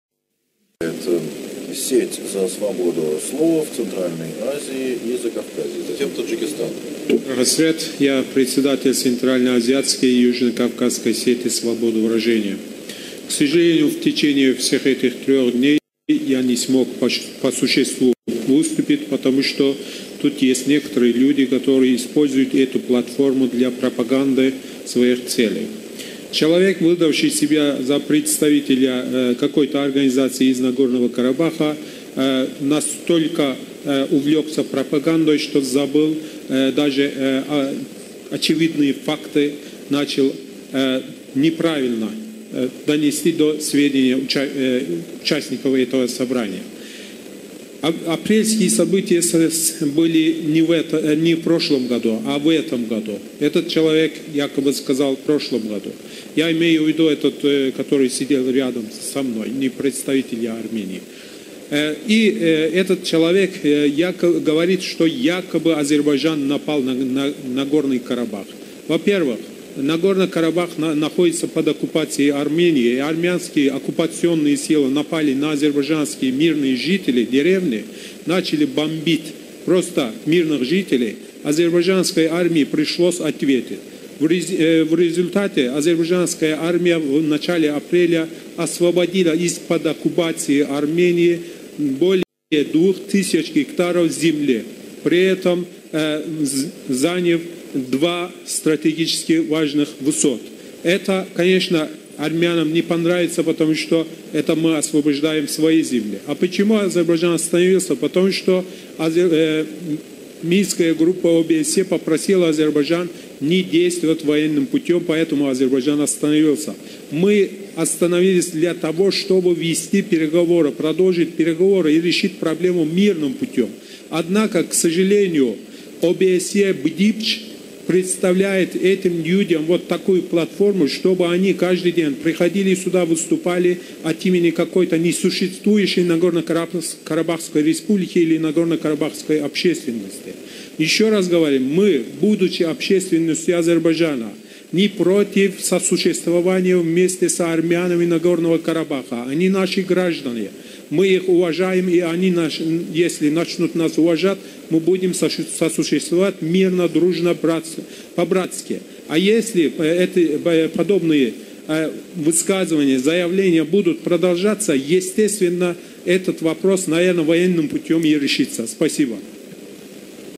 ОБСЕ БДИПЧ. Встреча по человеческому измерению - 2016. Азер Хасрет
Выступление Азера Хасрета на встрече по человеческому измерению ОБСЕ БДИПЧ. Варшава, 19-30 сентября, 2016 года. Видео: ОБСЕ БДИПЧ